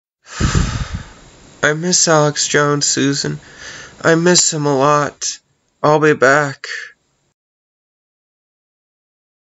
I Miss Alex Jones
It's been a hard time for us over on YouTube without Alex Jones. Susan doesn't like him, and our memes have suffered. I guess they don't want somebody who was talking about Epstein (who didn't kill himself) for over a decade.

I guess I'll have to watch his videos on banned.video

I like bittube.tv Has a built in cryptocurrency called Tube, and you can upload videos, pictures, and music without censorship.